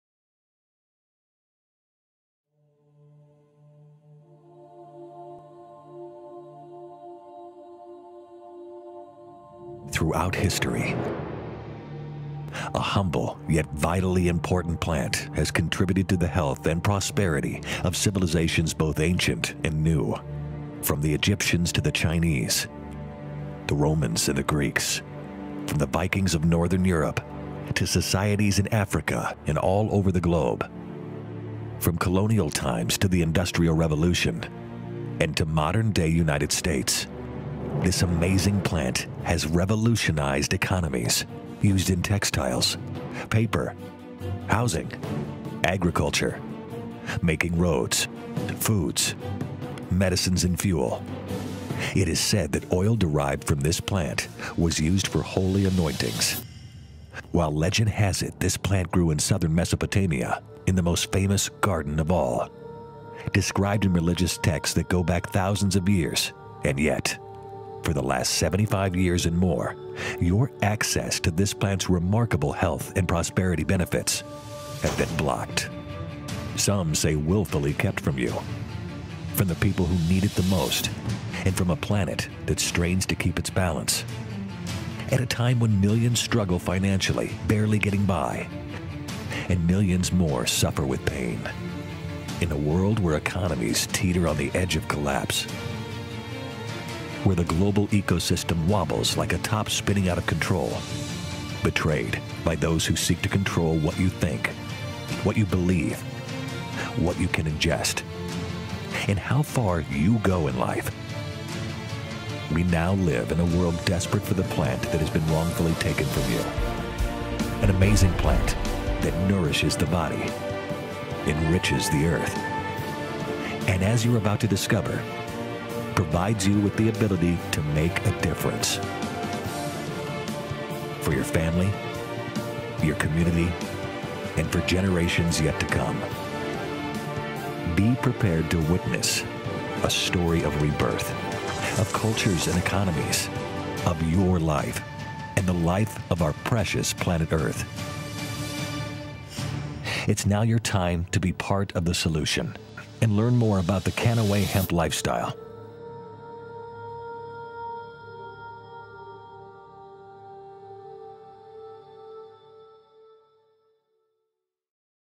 Kannaway ™
American used to depend on the Hemp plant, everyday! What happened? Learn what the government did with the Hemp Plant? Why is it illegal now with the Hemp plant? Let’s bring back the Hemp to America! What is Kannaway ™?